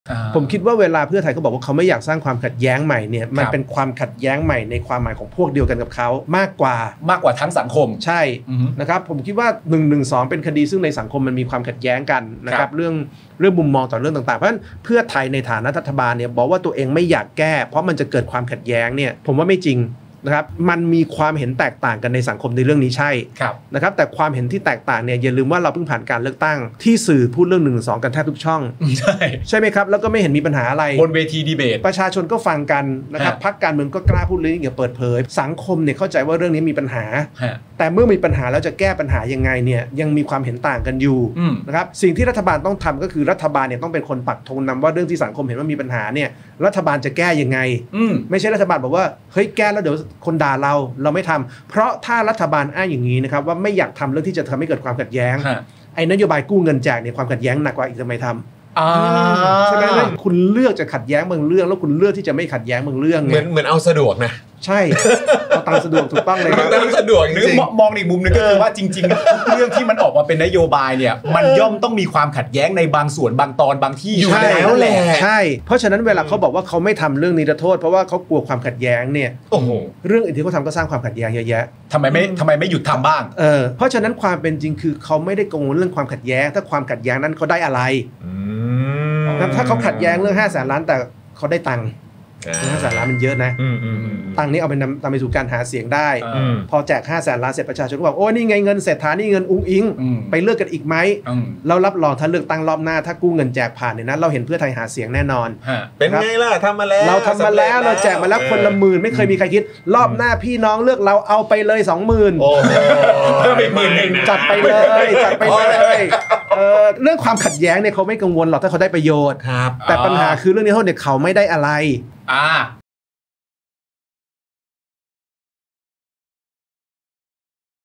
ไม่อยากสร้างความขัดแย้งใหม่แบบเพื่อไทย ความขัดแย้งของใครเหรอ?
สนับสนุนค่าเข้าชมรายการที่นี่ครับ

ศิโรตม์ คล้ามไพบูลย์ คุยนโยบายเรือธงของรัฐบาลเศรษฐา แบบตาสว่างเอกซ์ตราพลัส